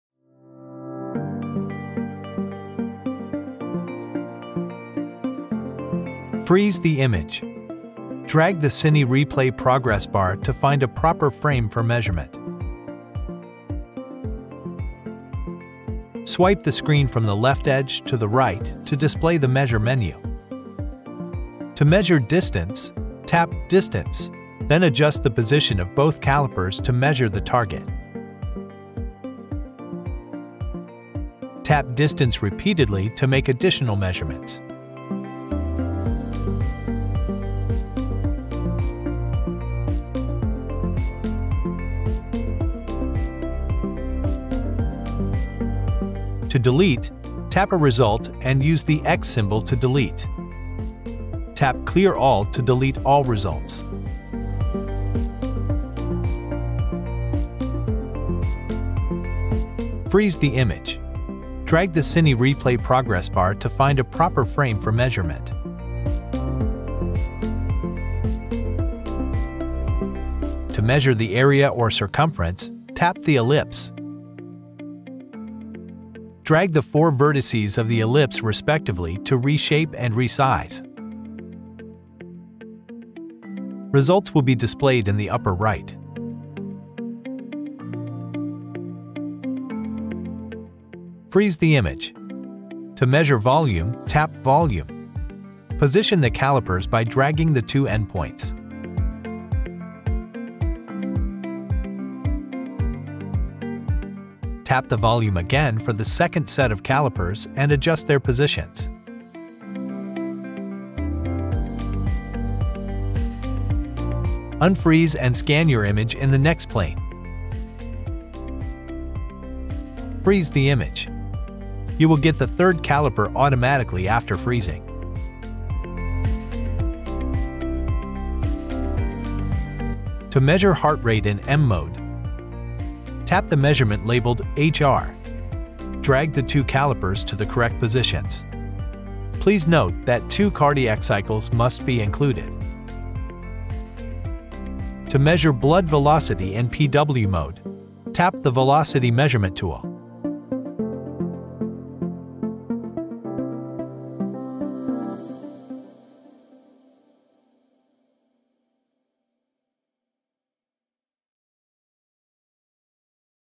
Mindray TE Air Tutorial - Basic Measurement
In this video, we'll guide you through the process of taking a basic measurement using Mindray's TE Air handheld ultrasound, a cutting-edge tool in medical imaging.